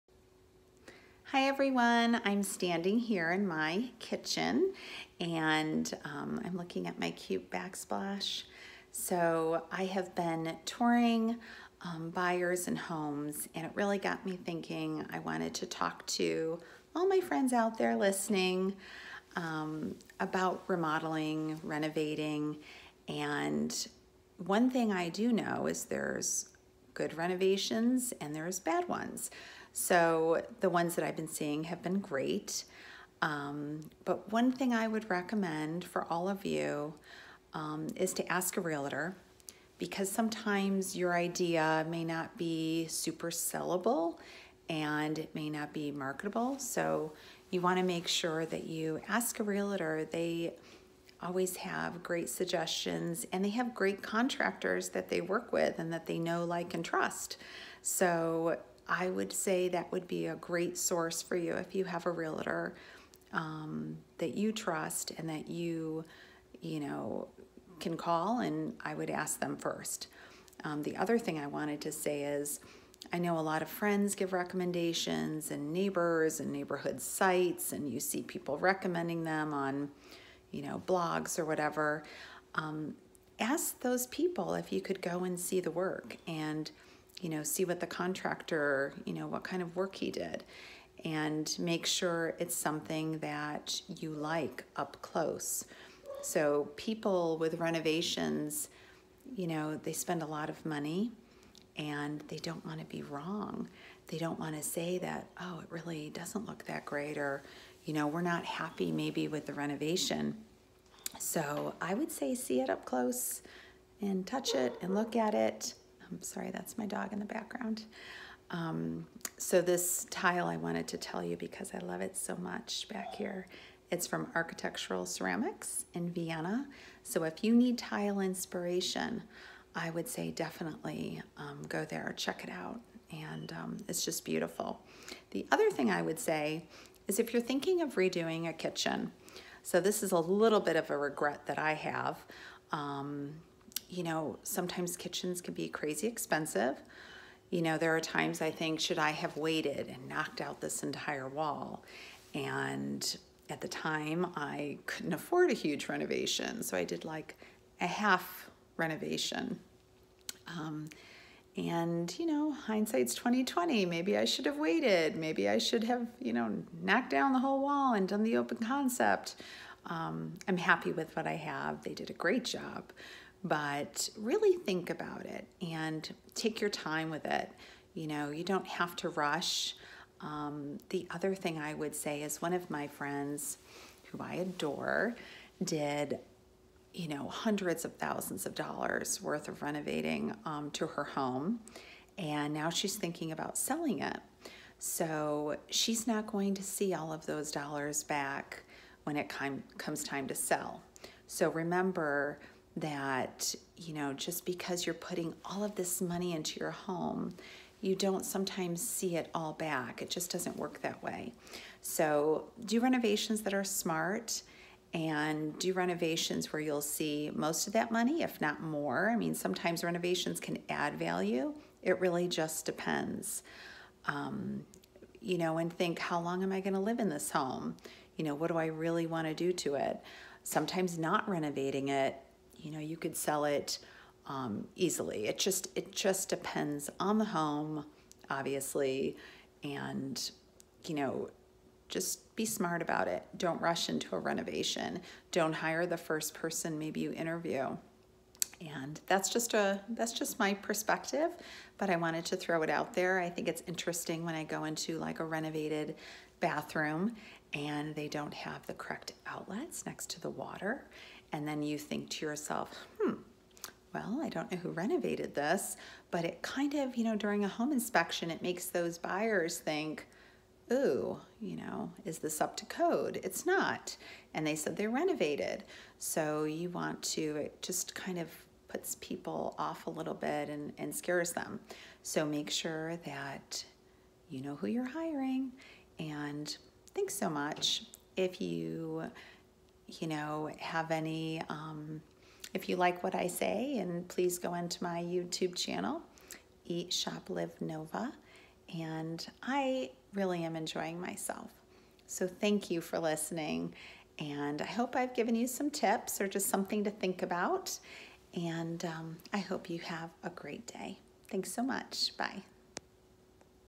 Don't Start Remodeling Until You Watch This Video with Realtor Kelly Pappas
Thinking of remodeling your home? Your first resource is your Realtor! They usually know amazing contractors.  Looking for tile inspiration? Check out Architectural Ceramics in Vienna, Virginia. Happy Remodeling!